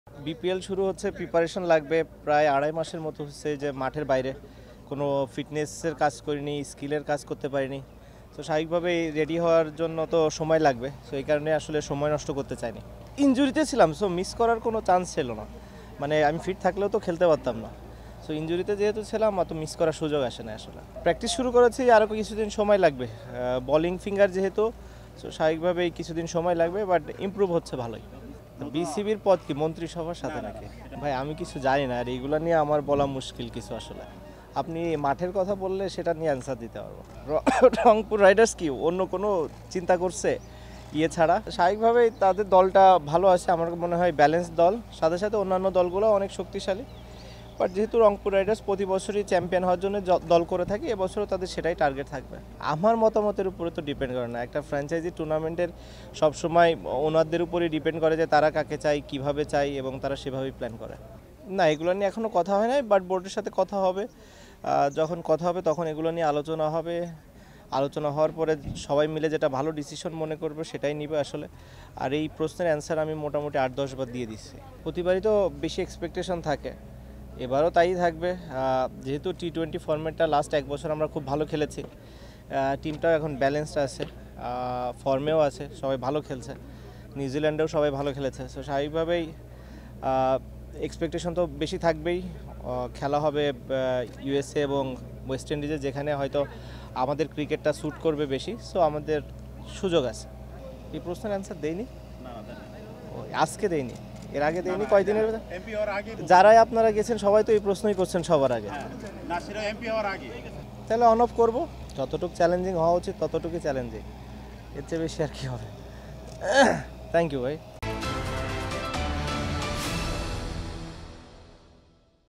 এমপি হওয়ার পরদিনই মাঠে, কারণ জানালেন সাকিব | Shakib Al Hasan | Election News | Mirpur Stadium
»» Office Address:
42, Shaheed Sangbadik Selina Parveen Sarak, Malibag 1217 Dhaka, Dhaka Division, Bangladesh